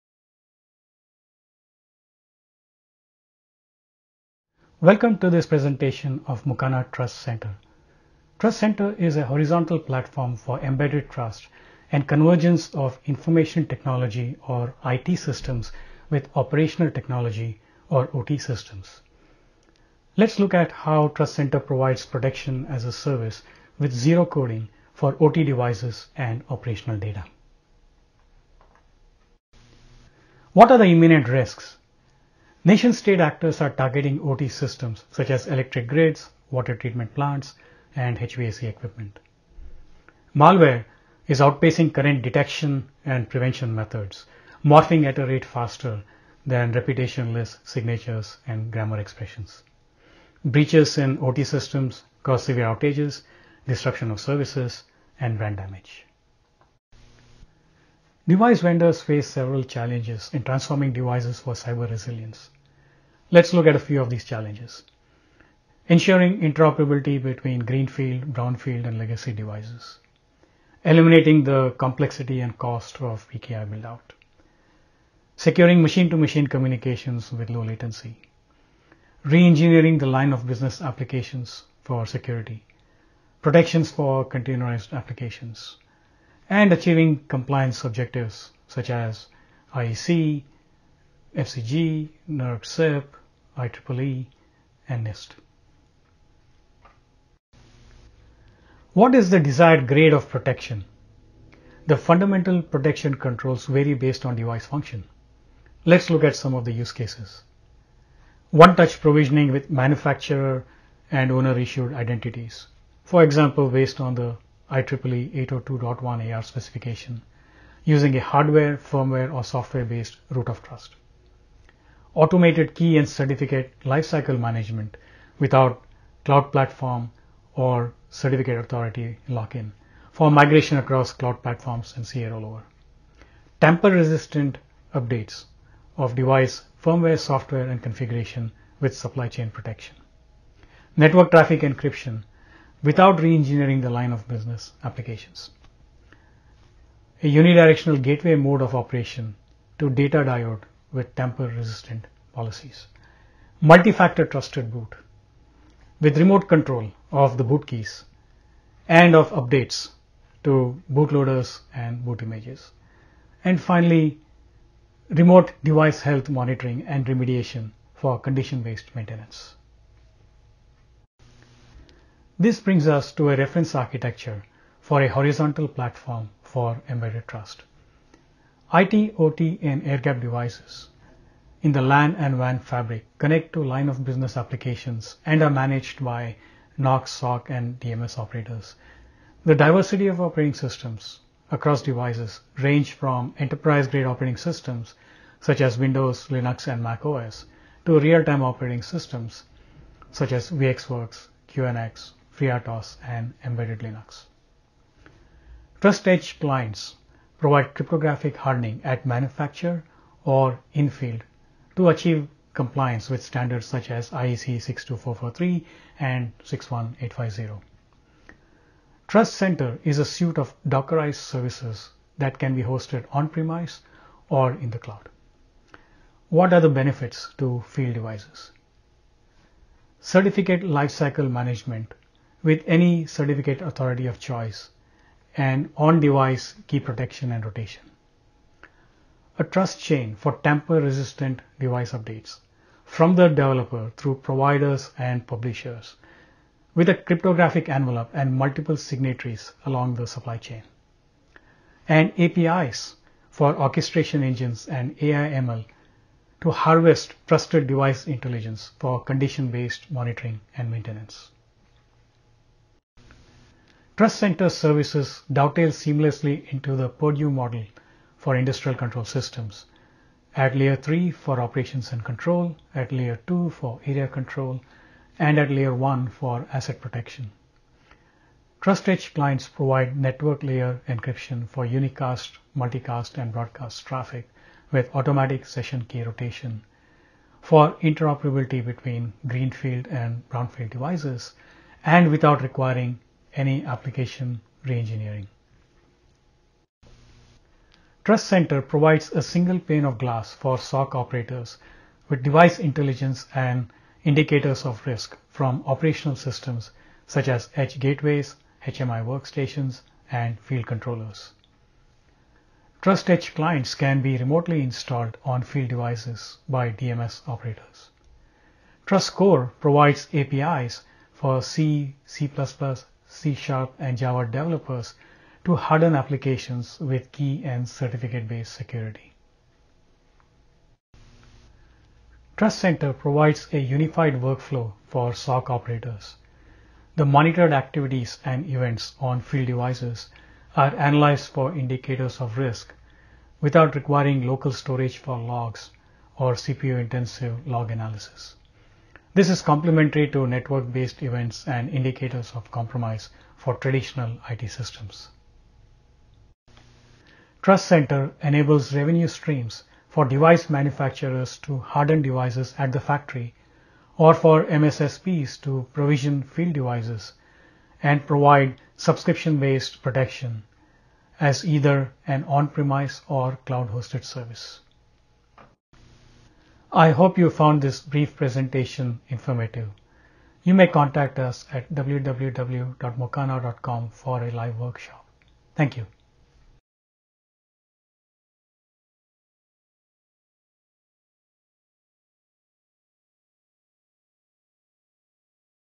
Mission Critical Protection: Secured Edge Device Communications
Get industry insights and learn technical details about Mocana's data protection and cybersecurity applications for edge device communication. Mocana offers cyber protection as a service through its software solution suite, which includes TrustCenter, TrustEdge and TrustCore.

Mocana solutions enable simple and scalable development & integration. The company's software system features an operations platform that enables tamper-resistant device lifecycle management with zero-coding, plus a development platform to harden and analyze device functions with minimal to no reengineering of applications.

to schedule a live solution briefing and explore key use cases as well as Mocana’s unique capabilities to address them.